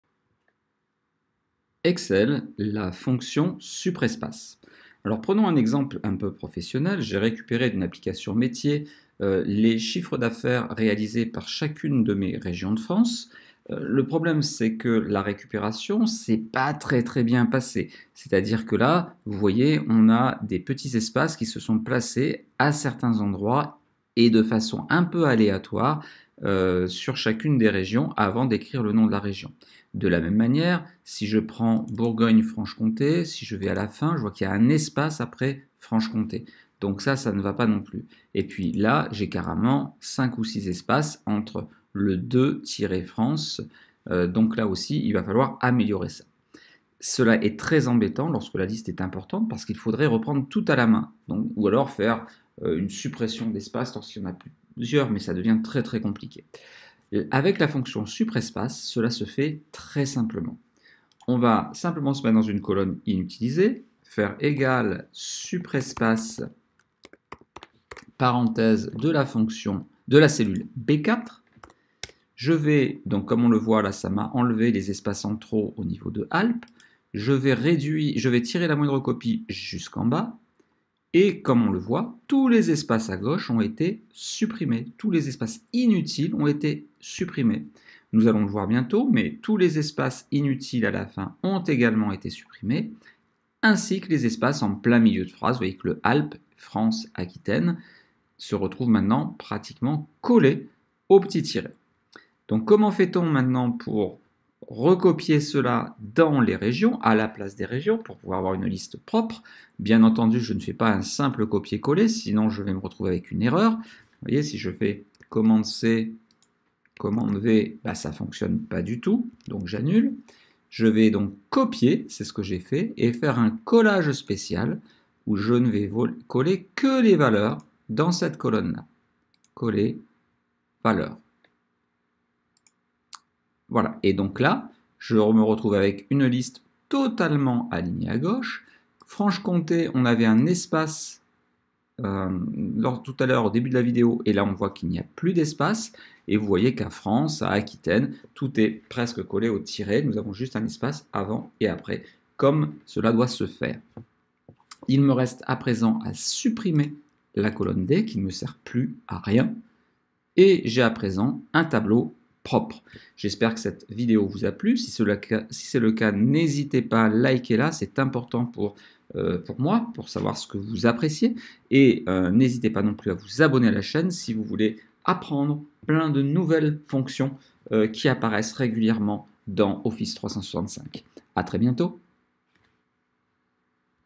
Comment supprimer les espaces inutiles dans des cellules Excel ? (XL05-excel016)
La fonction SUPPRESPACE() permet de supprimer les espaces inutiles dans une chaîne de caractères.